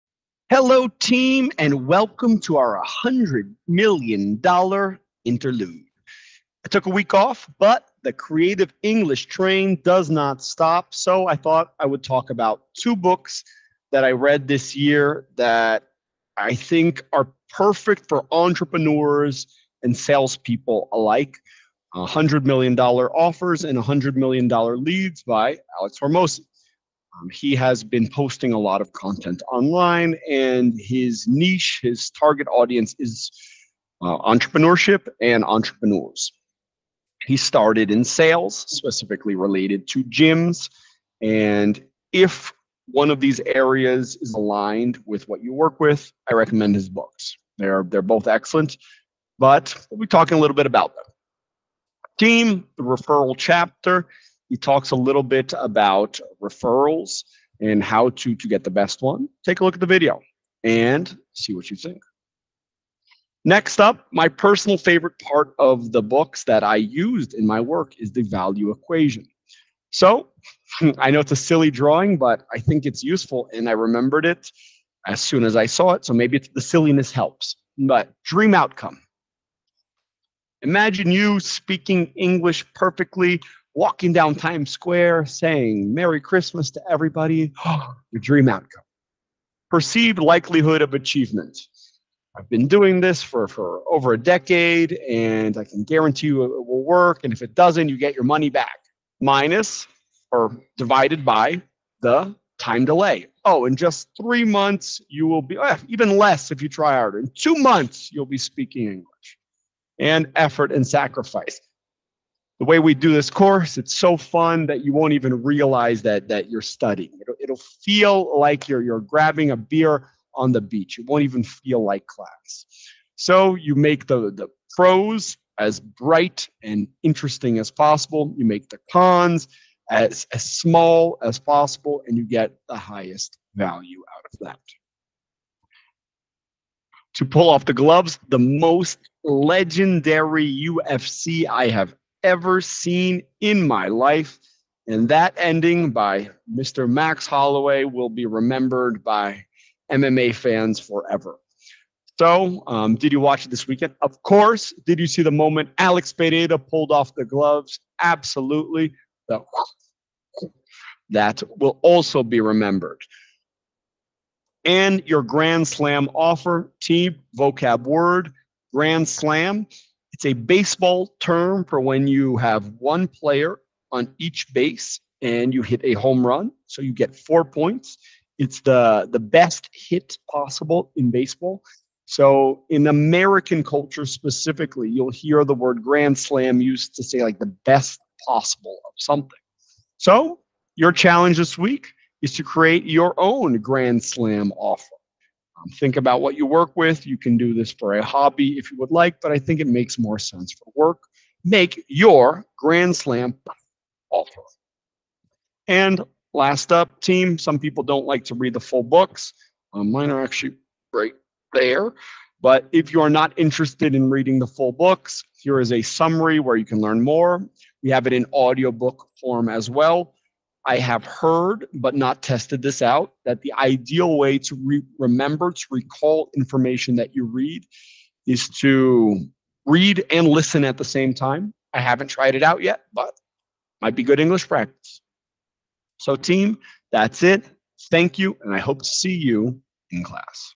Creative ‘24 E16: $100M book series review.
Introduction: I took a week off, so this will be a quick episode. Since many of my students will never see it, I get to pick a topic that has interested me this beginning of the year, entrepreneurship.

Seeing my work through this lens has been a novel perspective for me and one person whose content has stood out was Alex Hormozi. His no-excuse attitude towards work resonated with me and I would like to share some of the insights I’ve gleaned from his books: $100M Offers and $100M Leads and online content.

If you’re an entrepreneur or have a project you are looking to grow, this class is for you.

Themes: Alex Hormozi | Activity | Entrepreneurship